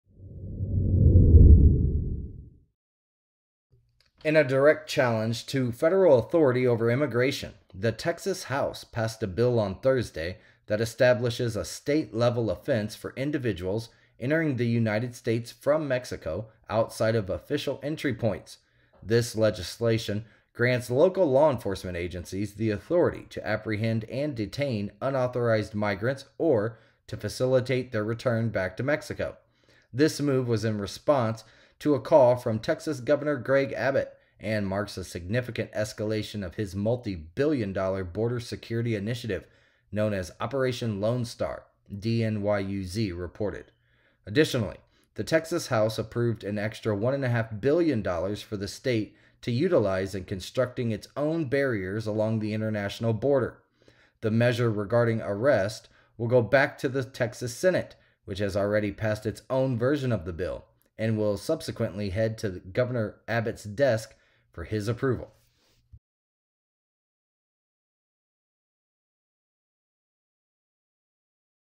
TX Lawmakers Vote to Let Local Police Arrest Illegals
In a direct challenge to federal authority over immigration, the Texas House passed a bill on Thursday that establishes a state-level offense for individuals entering the United States from Mexico outside of official entry points.

This legislation grants local law enforcement agencies the authority to apprehend and detain unauthorized migrants or to facilitate their return to Mexico.

This move was in response to a call from Governor Greg Abbott and marks a significant escalation of his multibillion-dollar border security initiative, known as Operation Lone Star, DNYUZ reported.